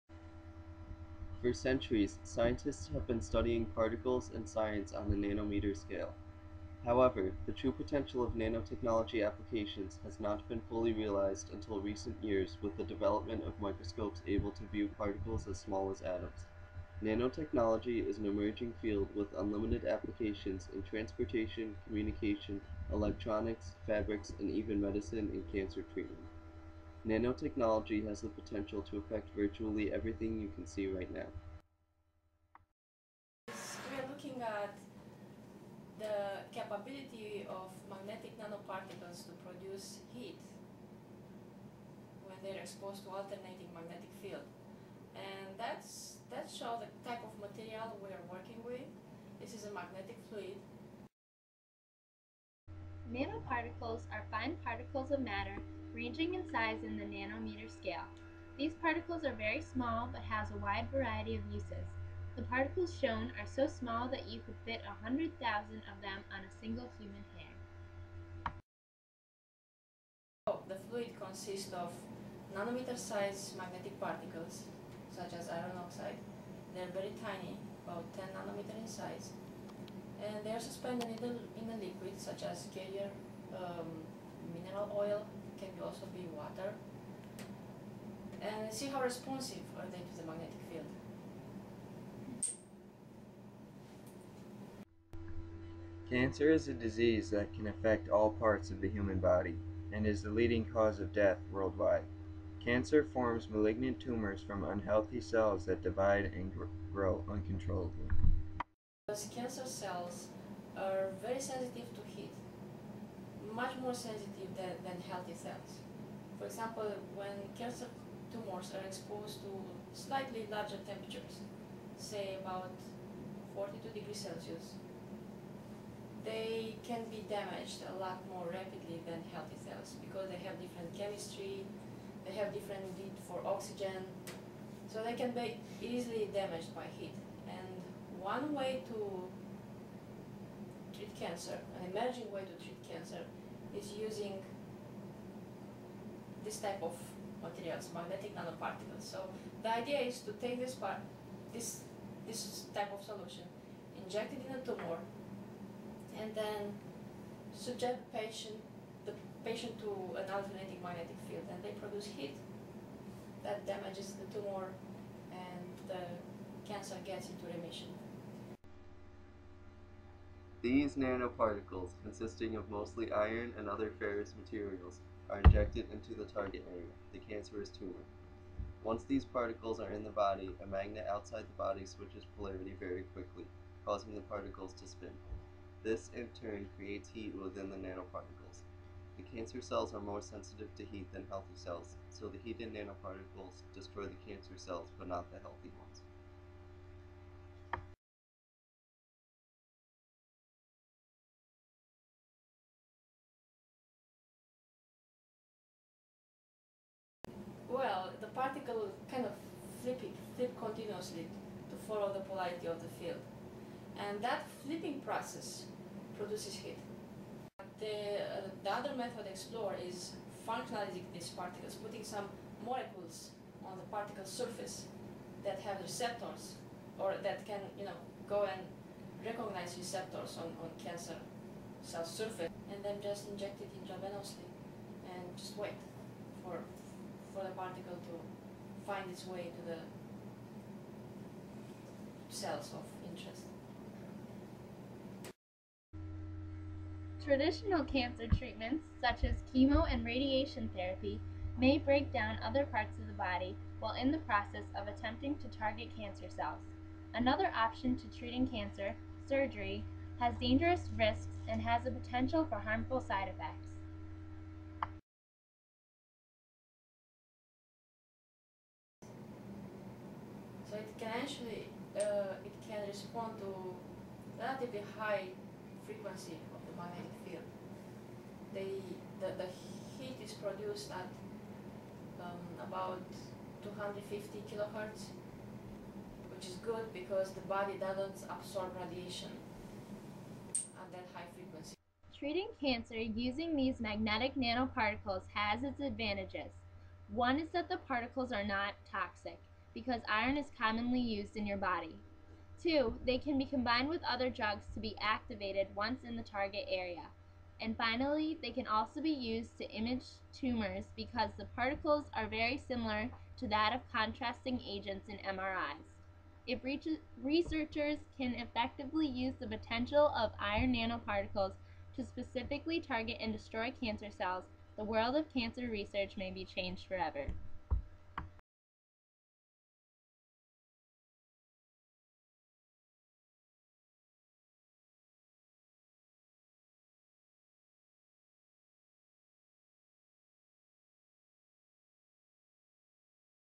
Magnetic Nanoparticles final.wmv
Questar III New Visions STEM students created this video for a high school project